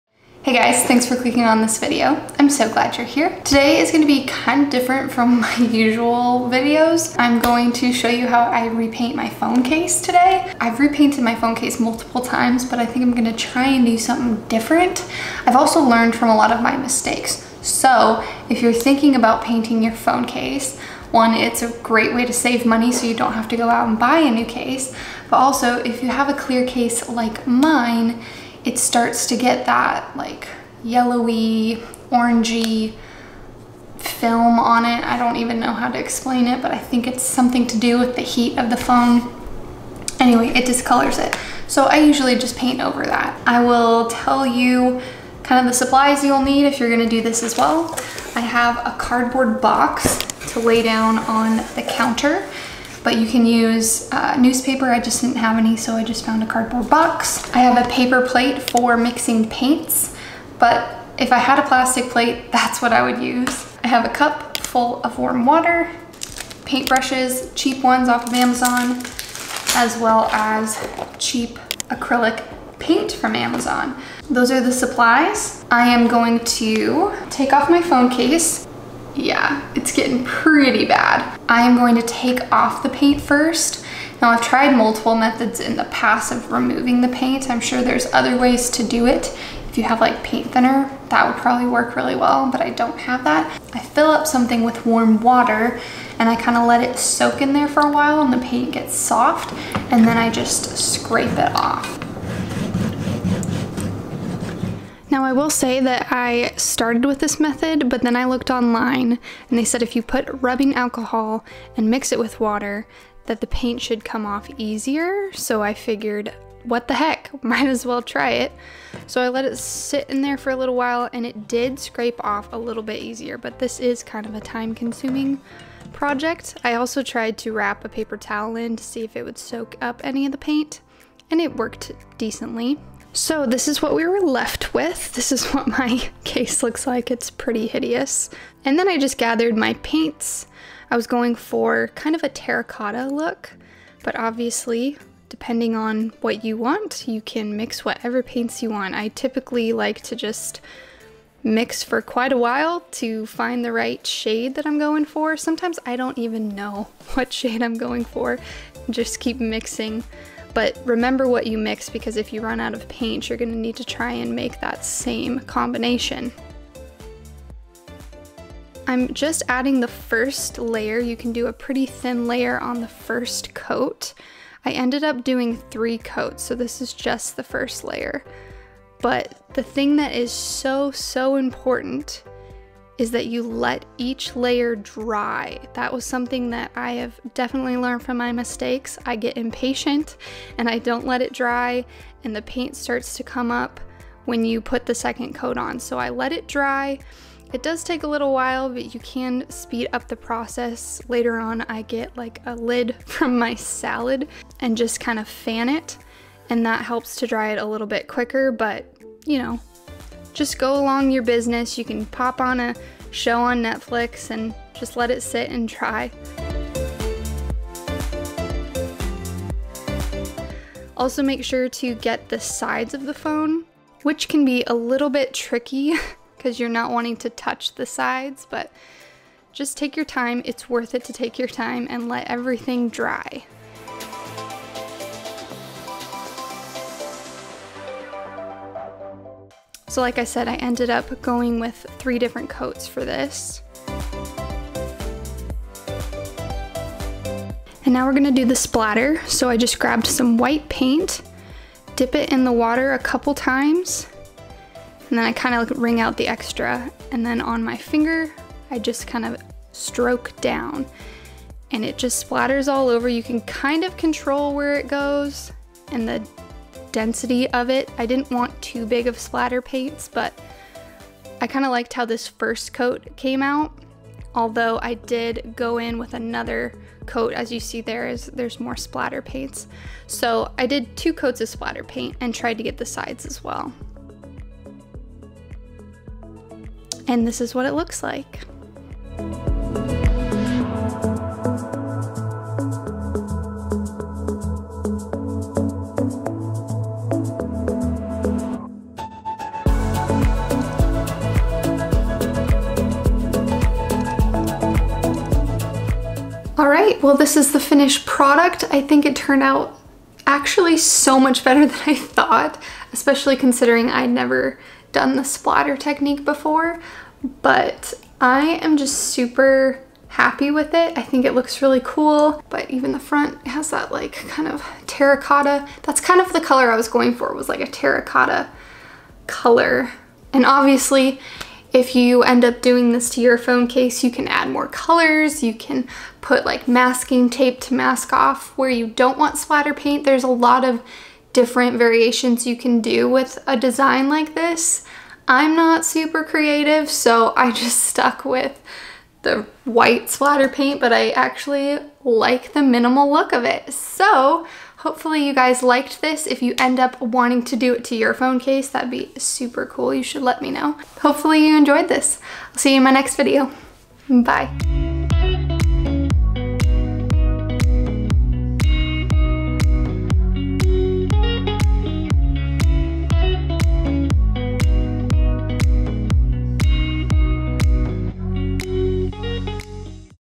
HOW TO PAINT YOUR PHONE CASE || Splatter Painting Technique with Acrylic Paint
hi friends!

how to paint your phone case & remove old paint!

Hope you like this DIY video about how to paint your phone case with acrylic paint and the splatter paint technique!

I've been painting my phone case for a while and tried this new splatter painting technique and it turned out so cool!

Do you paint your phone case?  Or will you try this technique out??

Hopefully you find this video helpful!

If you’d like more how to’s and diy’s on my channel, let me know :) this was fun to film!!

Much love,
Kendra